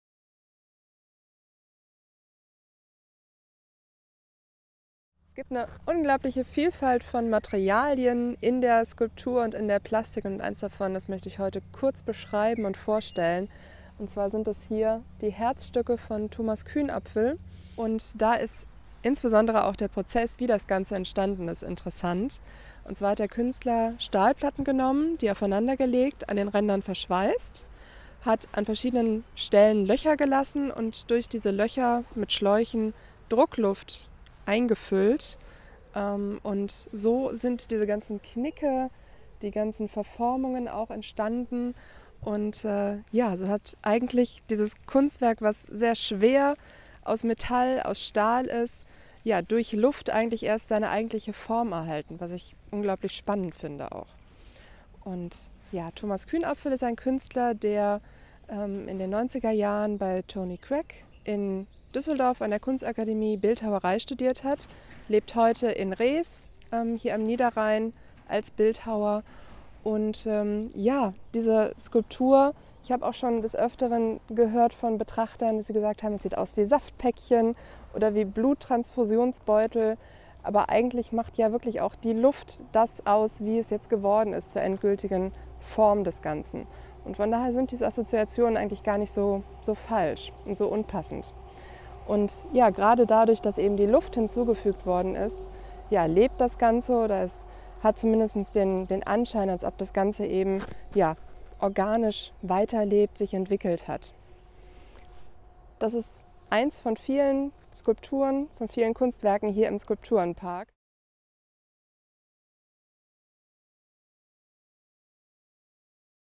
KuNNsttage - Sarah Lampe
Sarah Lampe, Leiterin Kunstvermittlung, zeigt in einer Reihe von Videos Skulpturen im Schlosspark Moyland und erzählt, welche Geschichten dahinter stecken. In diesem Video geht es um die Arbeit "Herzstücke" (1999) von Thomas Kühnapfel.